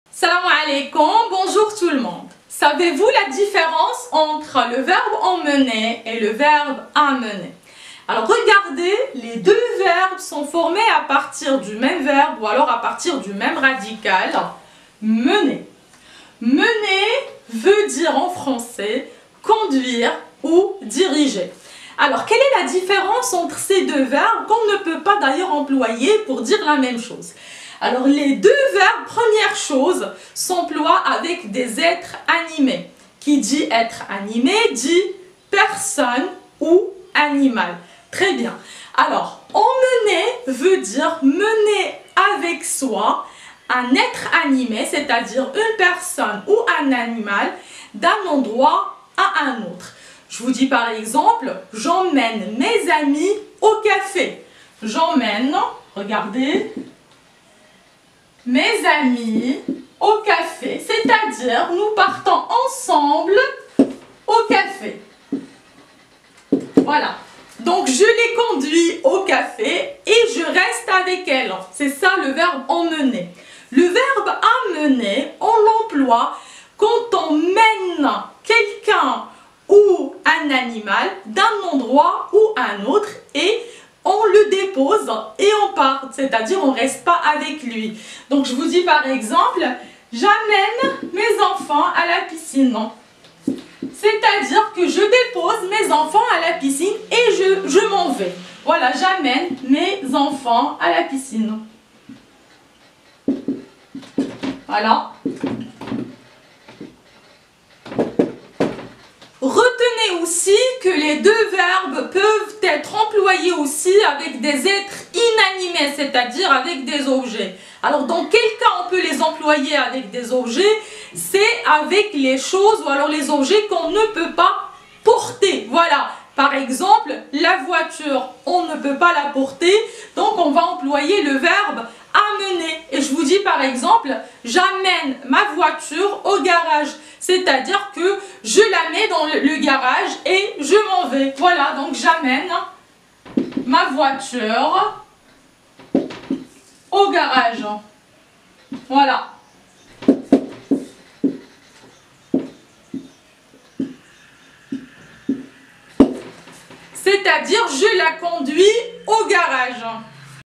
Quelle est la différence entre Emmener et Amener
Savez-vous la différence entre ces deux verbes: Emmener et Amener?
Si votre réponse est non, alors, n'hésitez à regarder la vidéo pour avoir la réponse.